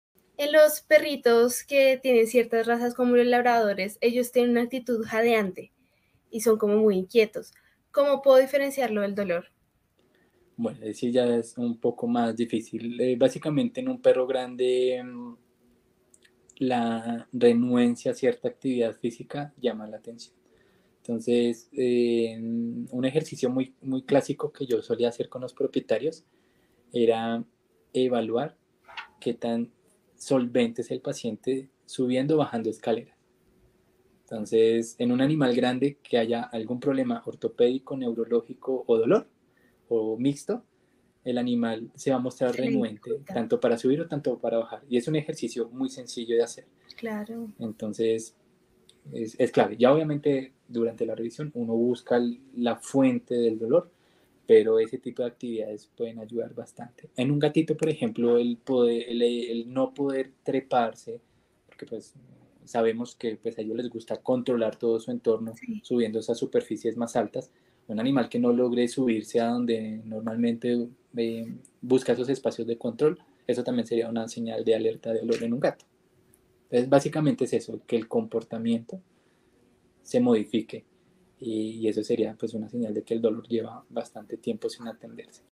¿Cómo Identificar el Dolor en Perros y Gatos? Consejos del Dr. Diego Ordóñez
¿Tu perro o gato evita subir escaleras o saltar a superficies altas? 🐾 Estas conductas pueden ser señales de dolor ortopédico o neurológico. Observa cambios en su actividad diaria y acude al veterinario si notas algo inusual.